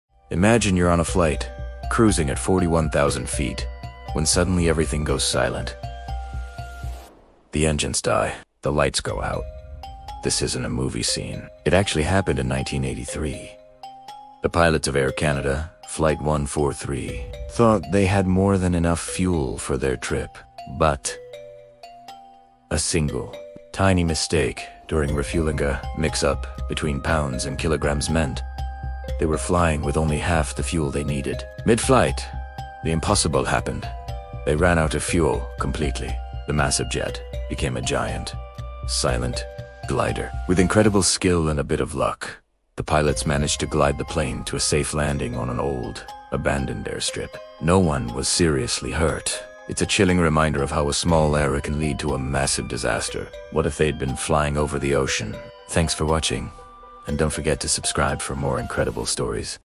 Human Error ✈️
One small calculation mistake nearly killed hundreds. A routine flight, trusted instruments, and a simple unit conversion error led to total engine failure at cruising altitude. This true story proves how fragile modern systems are—and how one human mistake can turn ordinary into fatal in seconds.